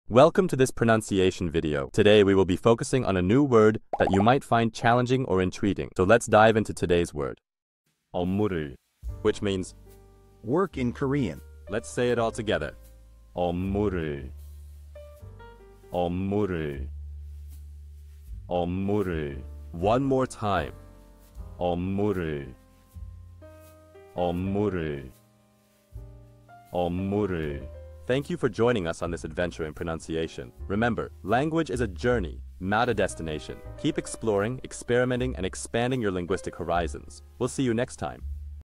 How to pronounce eopmureul | 업무를 (Work in Korean)
Master the Pronunciation of 'eopmureul | 업무를' - which means : Work in Korean 📝 with @PronunciationKorean 🎓🗣️ - [Your Guide to Korean]

Learn to pronounce 'eopmureul' perfectly in Korean with our easy and engaging tutorial by @PronunciationKorean! 📖📽️ In this comprehensive video, we provide you with native speaker examples 🗣️👂 and phonetic breakdowns 🎵 to ensure you get the pronunciation just right.